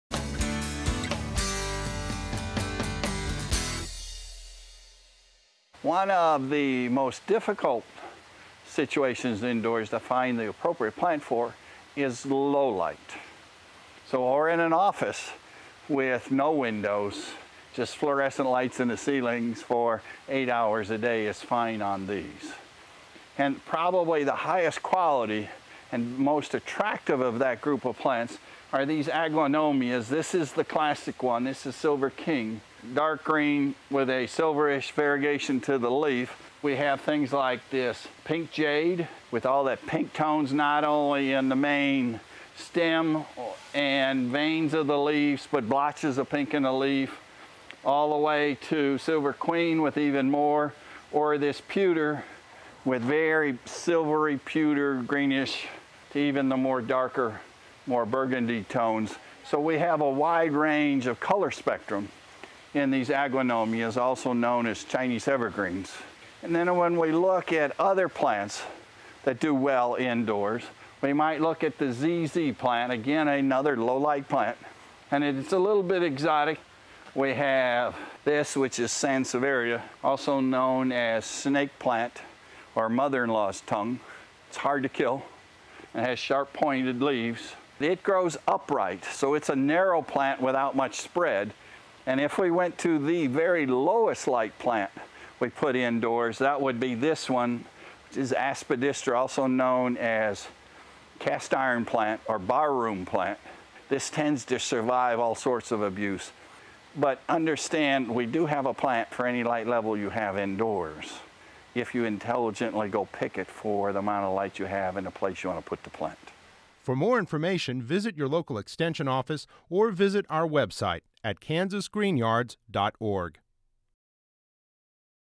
Low Light Indoors? No Problem!
The most important criteria in selecting a plant for indoors, to enhance our décor, is the amount of light that's ideal for the plant to grow in. This segment shows a wide variety of plants to choose from when decorating your home or office with low light levels.  Produced by the Department of Communications at Kansas State University.